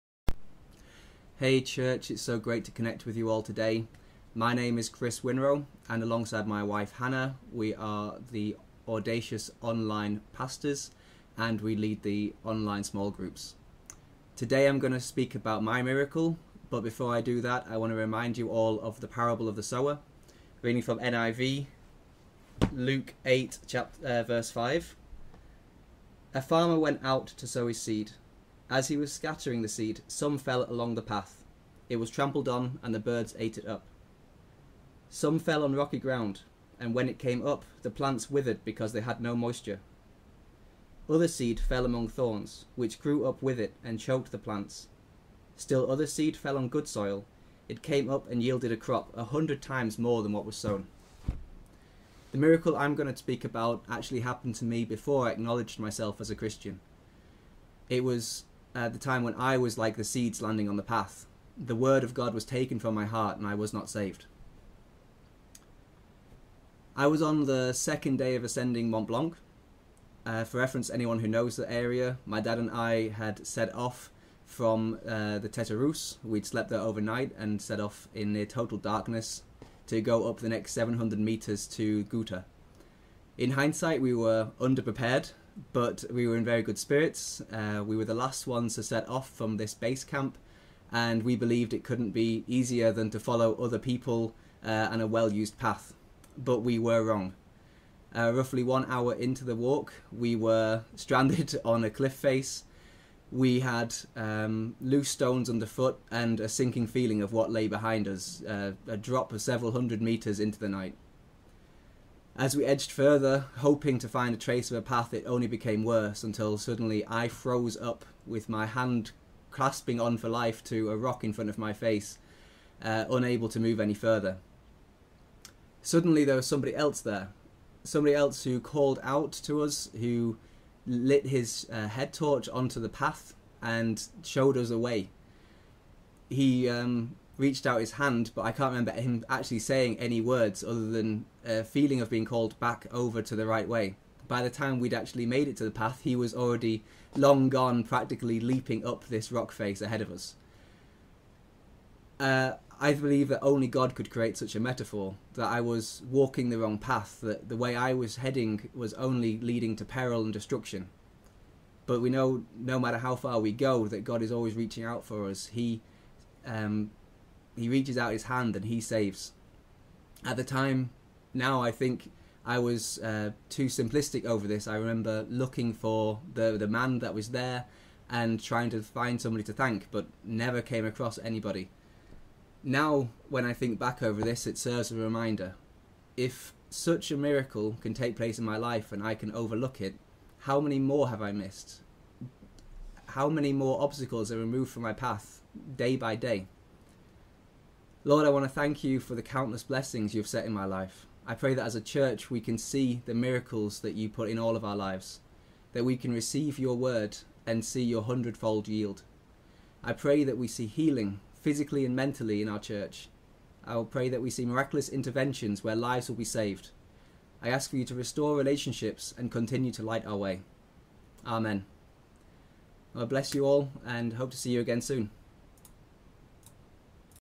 My Miracle - !Audacious Devotionals - Friday 20th January 2023
Today we continue our series of devotions which come from the other side of our week of prayer and talk about 'My Miracle'

In this series of devotions we hear from a number of people who share with us their story of change and provision after God had answered their prayers.
We continue our series with Chris Winrow who oversees our Online Campus.